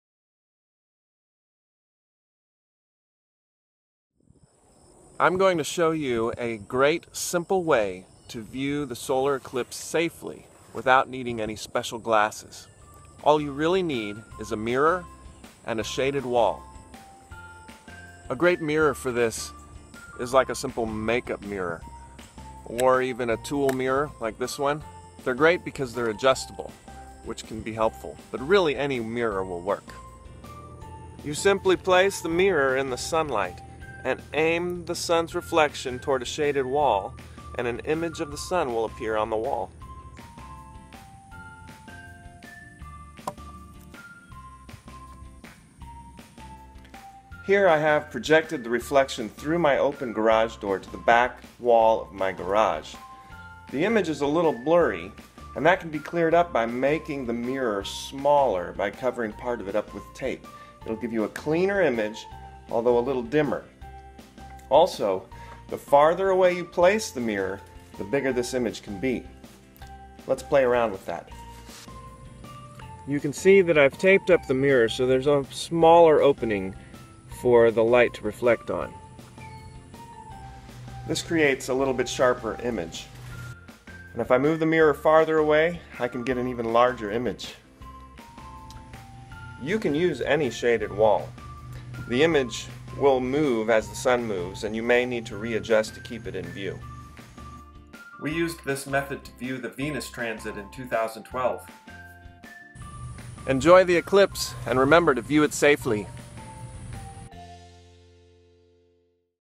Mirror Solar Viewer
A simple, safe way to view the solar eclipse without special glasses